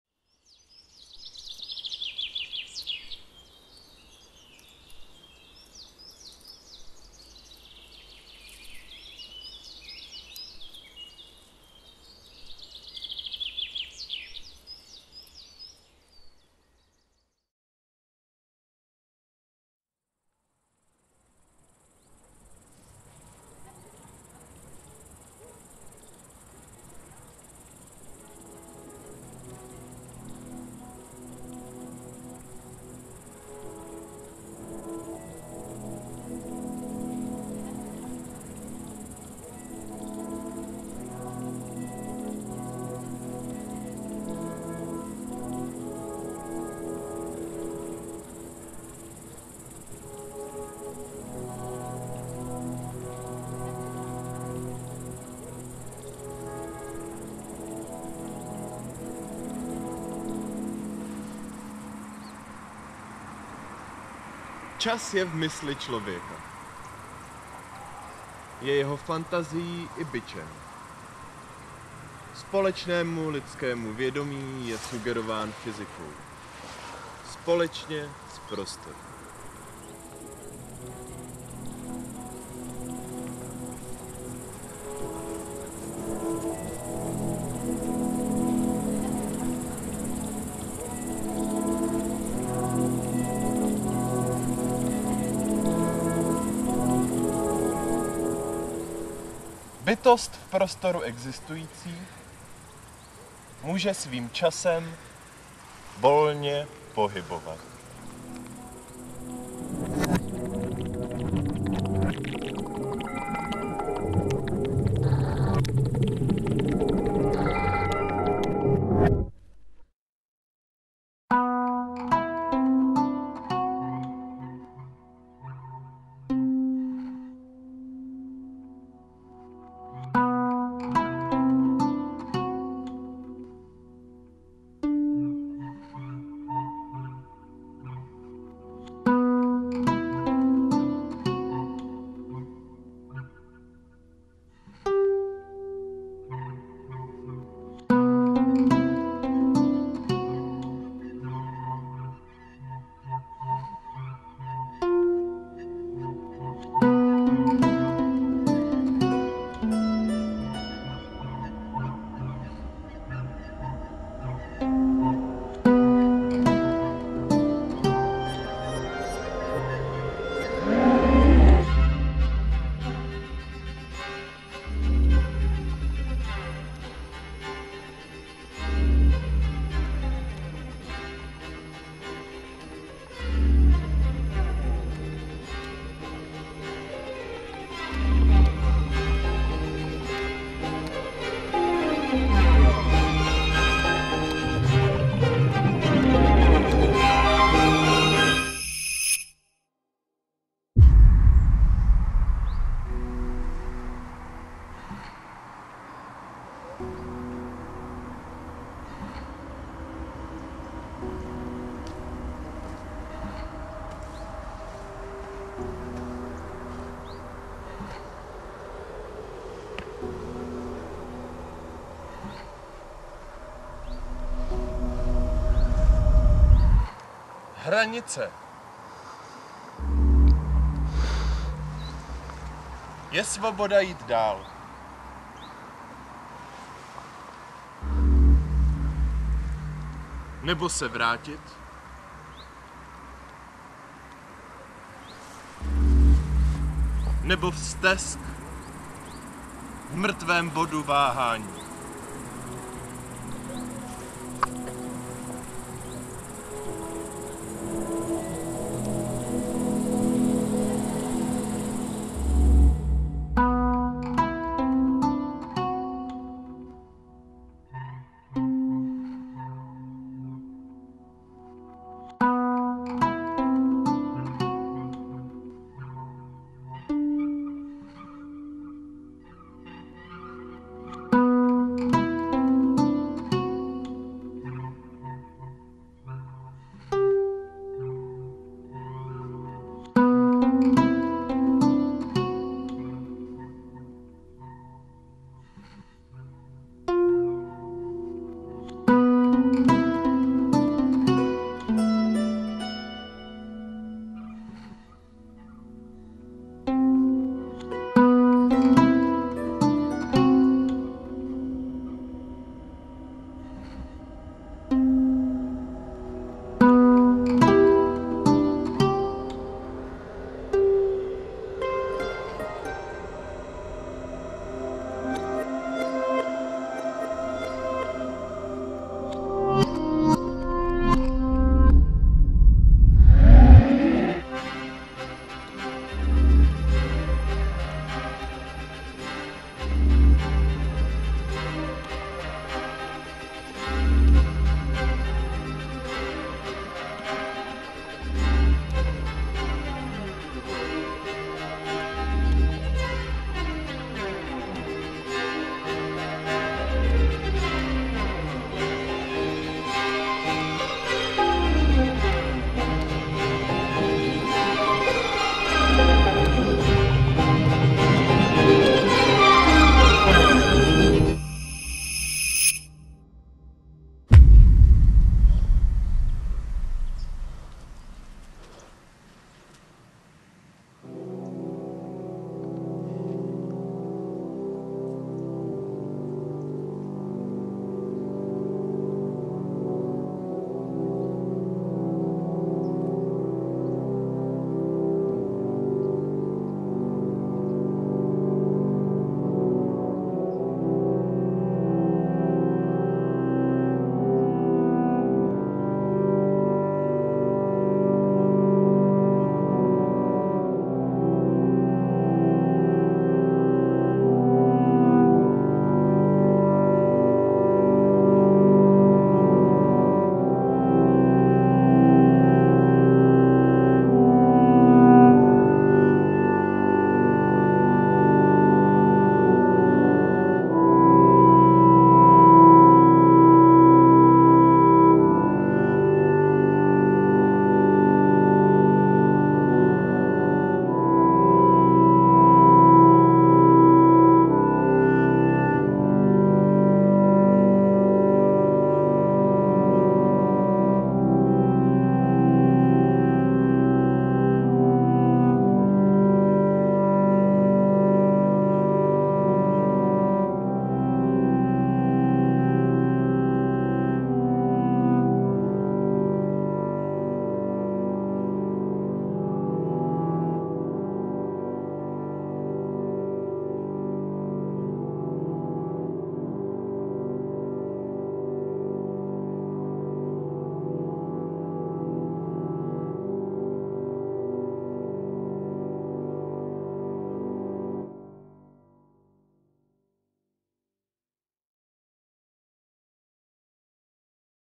Hranice / Borderline - beautiful dance in nature
Beautiful video from Czech Country from Vysočina, tribute to the place itself, author of the vision for this place and choreography: Vaclav Kovar from Loco:Motion Company.
Video has been made after 5 days spend on this exact place and looking for the only dance that can suit it. Tasking question about human existencion in time , bringing borderline between statement in nature and in human society.

Site-specific dance video
Music made by Cocojammin production (Adam Lanci)
supervision:Katerina Eva Lanci